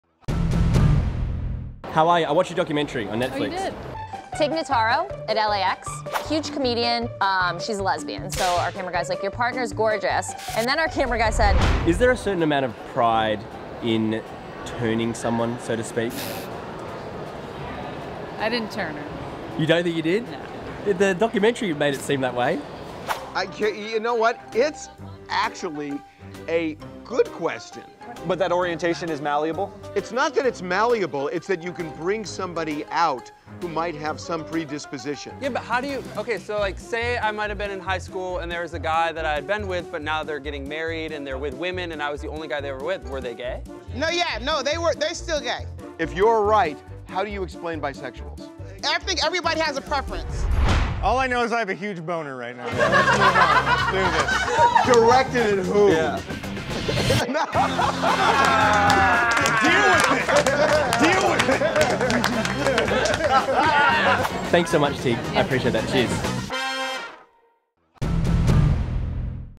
Comedian Tig Notaro Has A Very Gorgeous Partner! | TMZ
We talk to comedian Tig Notaro about her documentary on Netflix, which also stars her very gorgeous partner.  Our photog asked Tig about how much of a role did Tig have on her partner changing her sexual orientation?